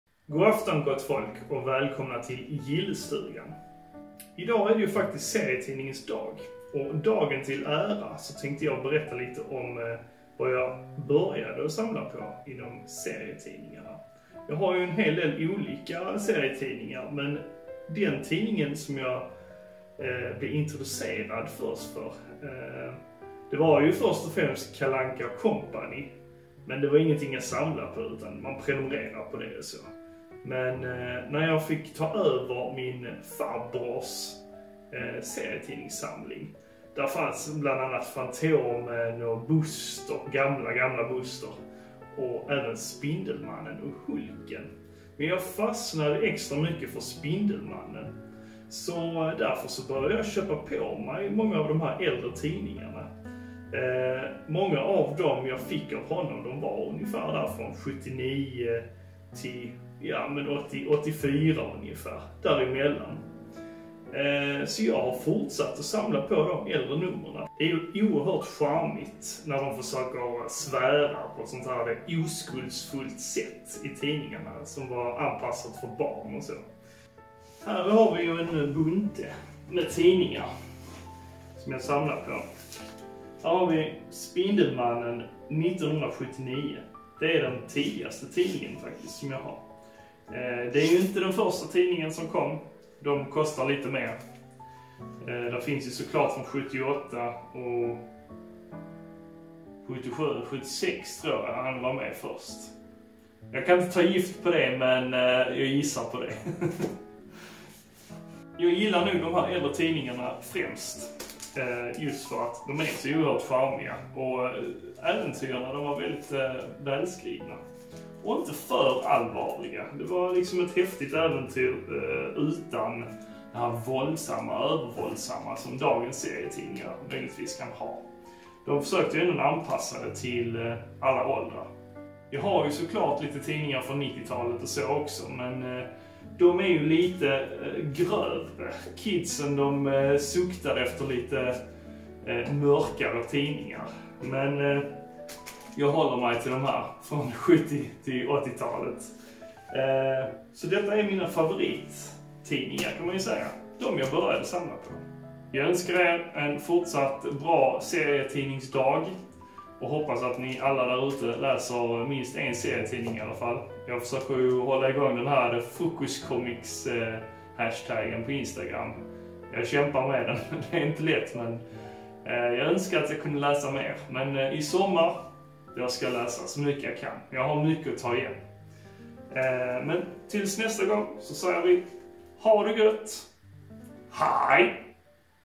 Serietidningens dag 2021
Niclas berättar om begynnelsen av sitt serietidningssamlande.